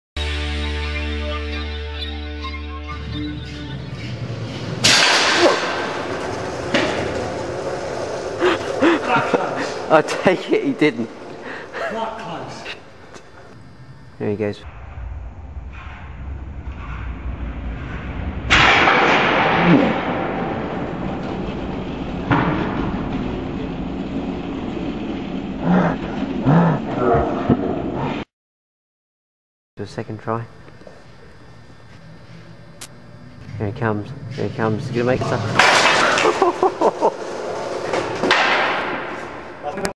Skateboarding Fail
This is an old bit of video of one of my very good friends trying to squeeze through a space that is obviously too small for his 6'4 frame.